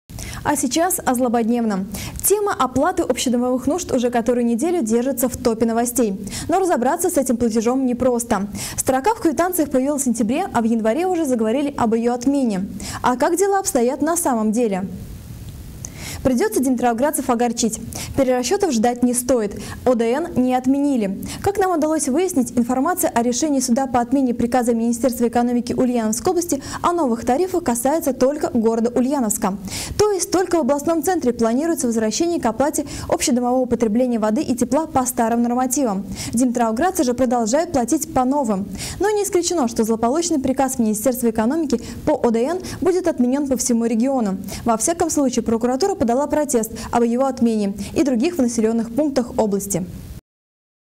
В Димитровграде оплата ОДН остается
Придется димитровградцев огорчить. Перерасчетов ждать не стоит. ОДН в нашем городе не отменили, сообщает